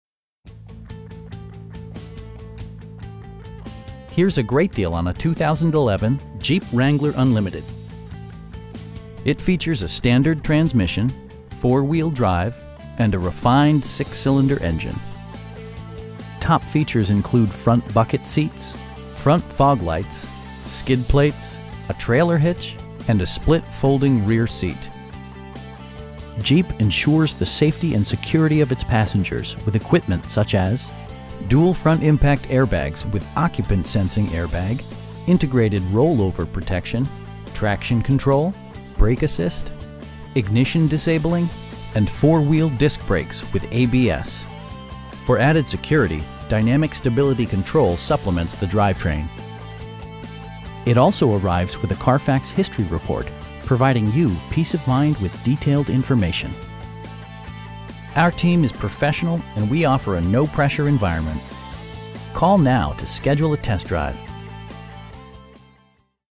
2011 Jeep Wrangler Unlimited Sport in Greenville, NC 27834
Barbour-Hendrick Honda Greenville
3300 S Memorial Dr

Here's a great deal on a 2011 Jeep Wrangler Unlimited. It features a standard transmission, 4-wheel drive, and a refined 6 cylinder engine. Top features include front bucket seats, air conditioning, front fog lights, tilt steering wheel, skid plates, cruise control, a trailer hitch, and a split folding rear seat. Jeep ensures the safety and security of its passengers with equipment such as: dual front impact airbags with occupant sensing airbag, integrated roll-over protection, traction control, brake assist, ignition disabling, and 4 wheel disc brakes with ABS. For added security, dynamic Stability Control supplements the drivetrain. It also arrives with a Carfax history report, providing you peace of mind with detailed information. Our team is professional, and we offer a no-pressure environment. They'll work with you to find the right vehicle at a price you can afford.